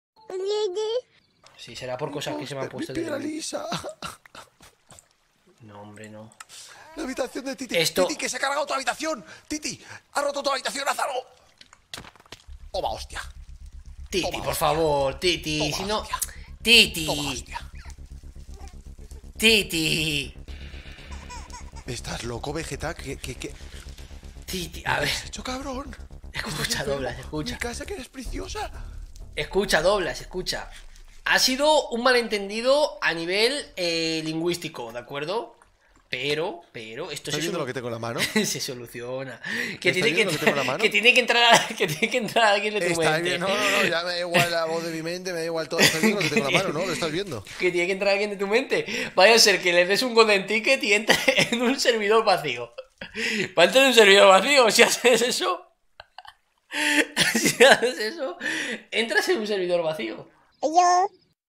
TITI MATA A VEGETTA POR DESTRUIR SU HABITACIÓN KARMALAND 5
No bajes por favor. :c